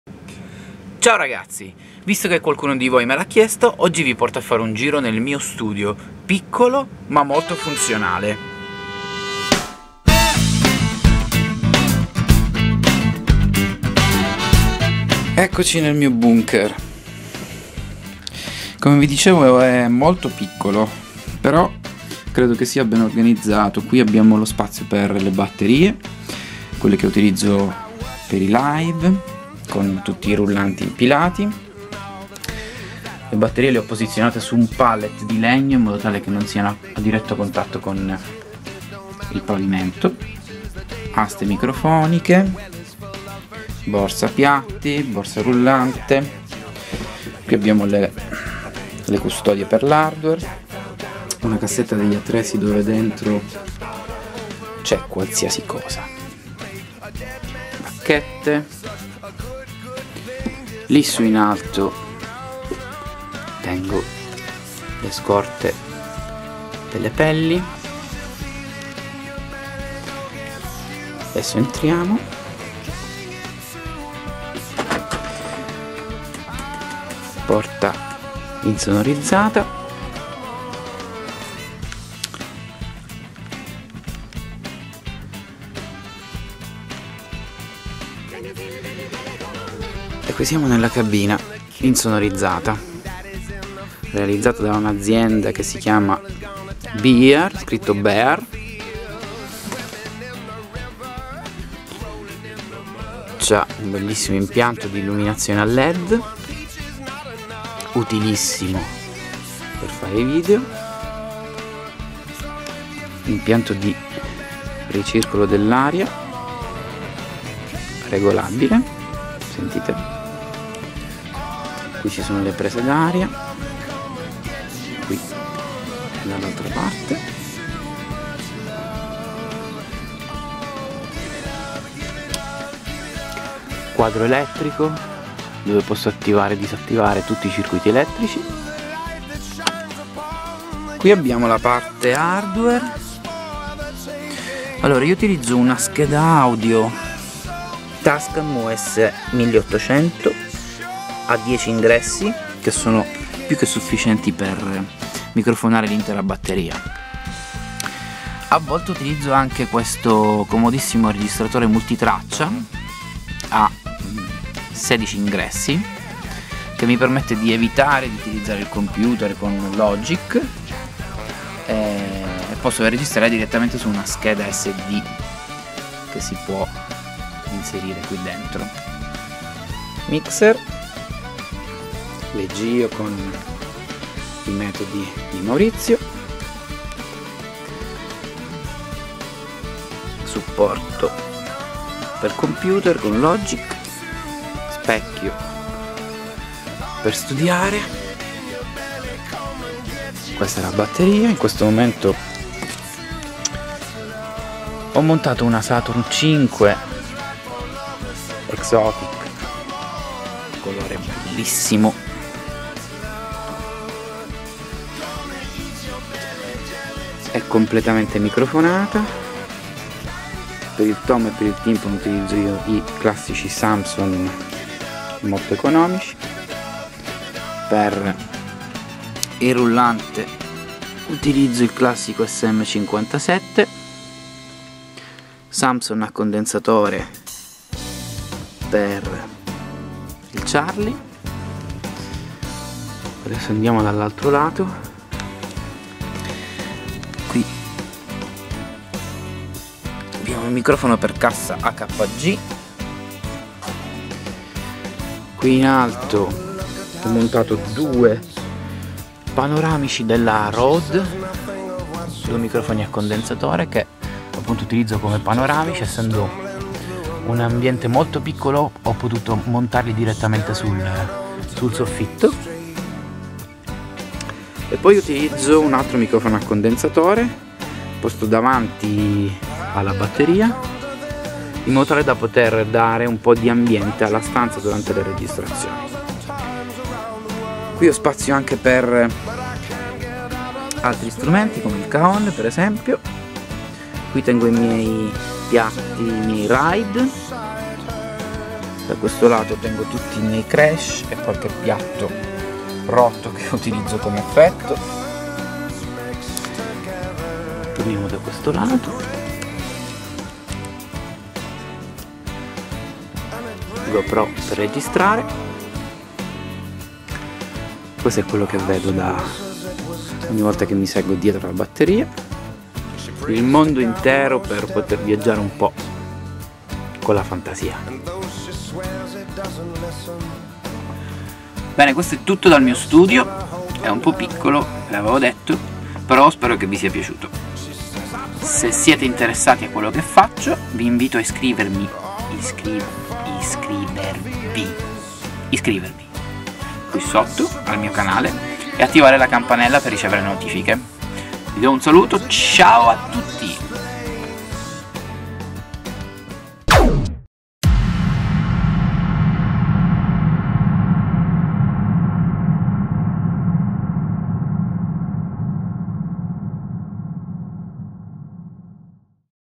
IL MIO STUDIO DI REGISTRAZIONE - Federico Collovà Drummer
Ecco a voi un breve giro del mio studio. Piccolo ma molto bene organizzato.
Dentro ho spazio per tutto ciò che mi serve per poter lavorare in modo professionale e con ottimi risultati sonori.

- microfoni samson (tom, timpano e hh)
- microfono rullante
- microfono cassa